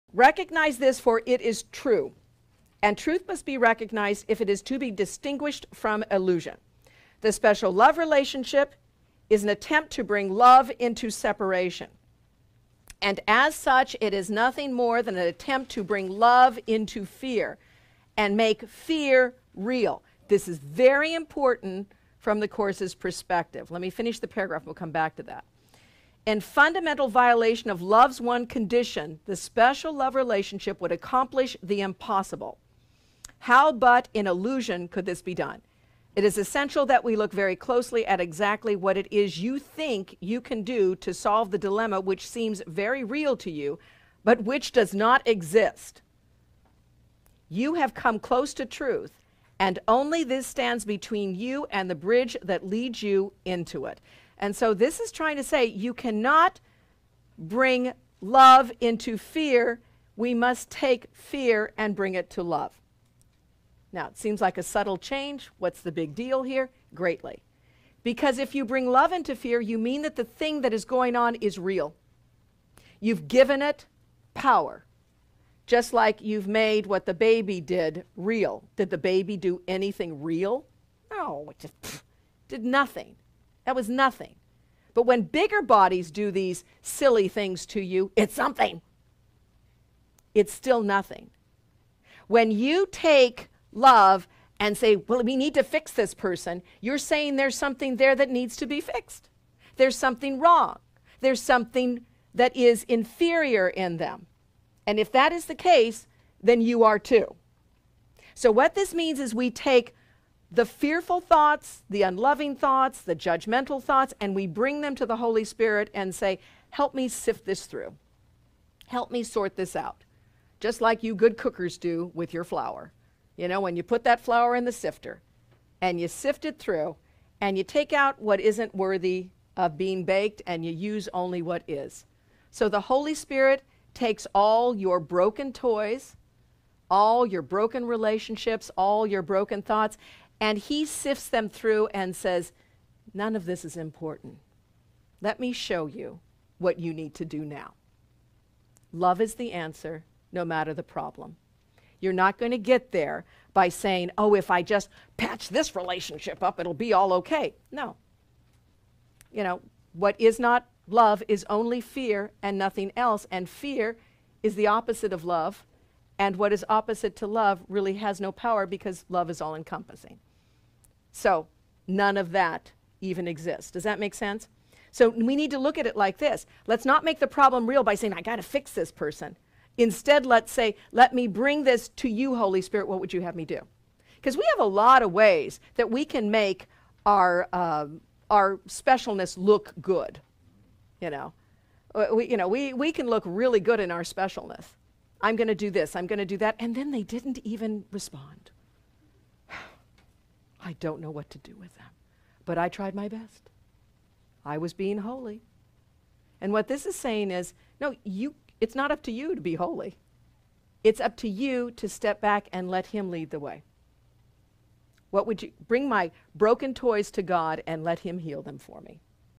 Beverly on A Course in Miracles - 164
Beverly Hutchinson McNeff's weekly study group on "A Course in Miracles." Watch FREE on Wednesday evenings at 7pm PT/10pm ET